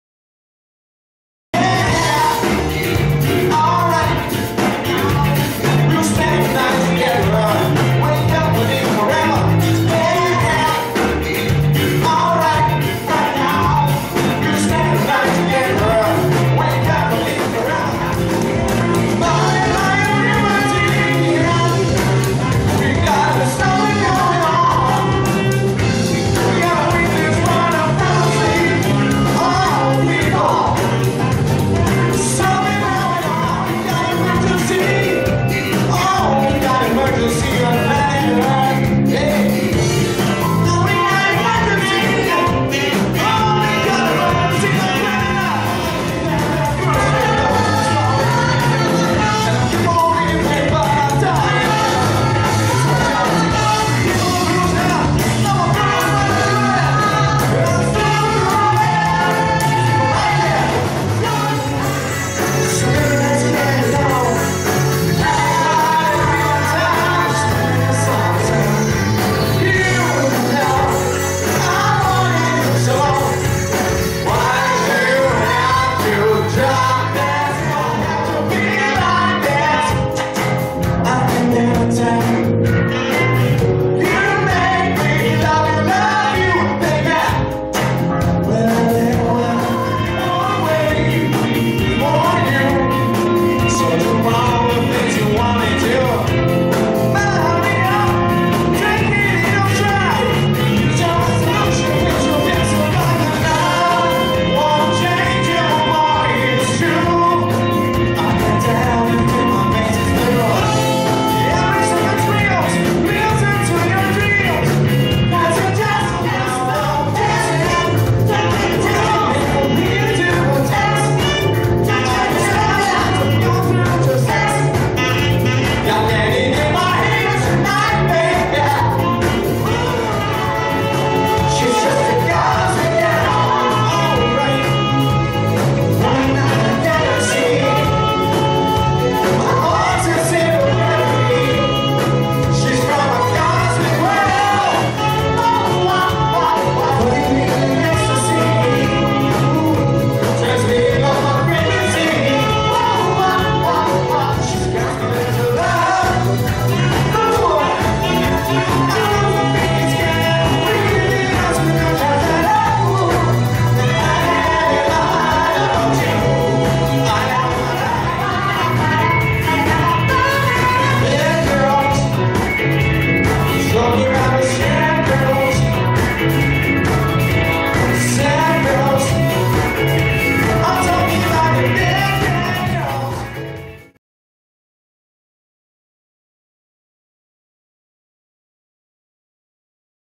Medley @ London Station (Jamiroquai tribute)
Medley do show realizado no London Station Pub (São Paulo - SP) em  2015 (formação enxuta em função do palco).